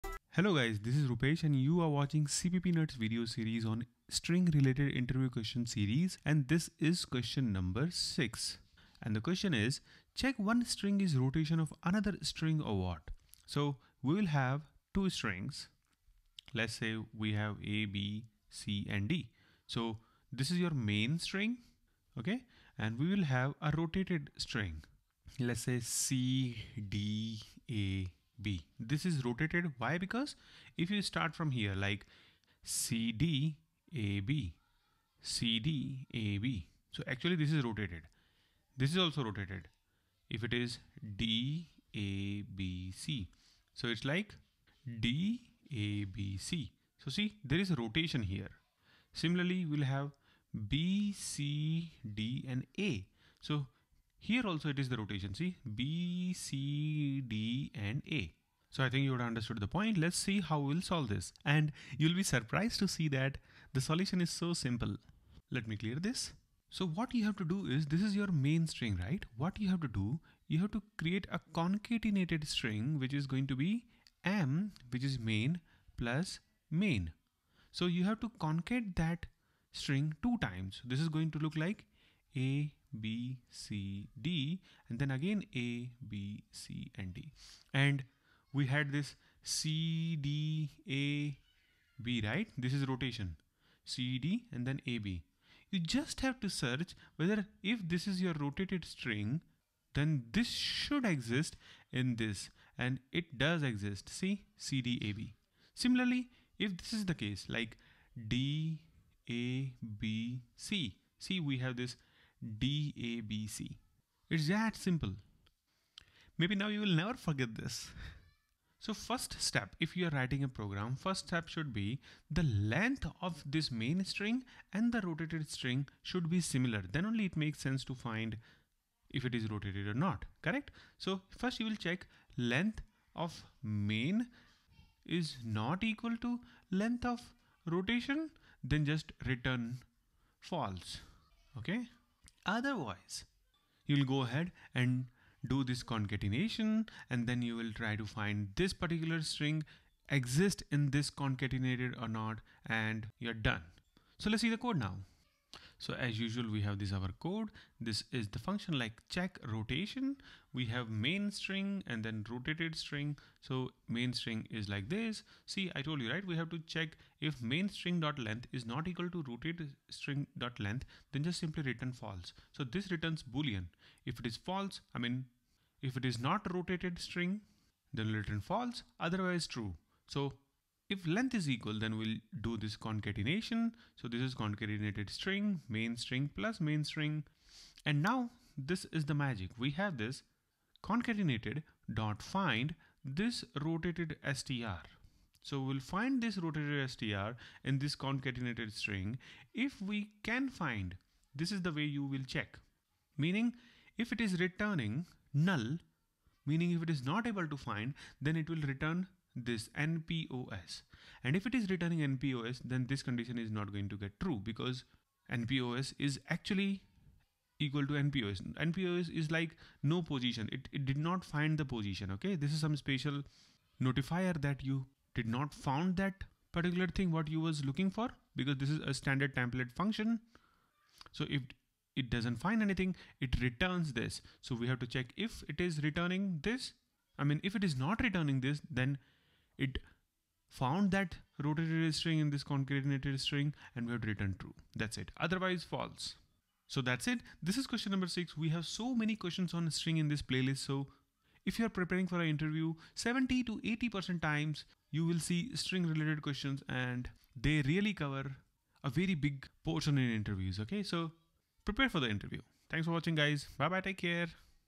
Check If One String Is Rotation Of Another String
JOIN ME

Time Lines
0:05 Introduction
1:11 Approach
2:40 Code Explanation

In this video we will learn about how to find one string is rotation of another string. This is one of the interview questions for computer science and electronics students.

Th solution to this problem is very simple, we just have to contact the main string and apply stl find function and see if we can find rotated string in main string.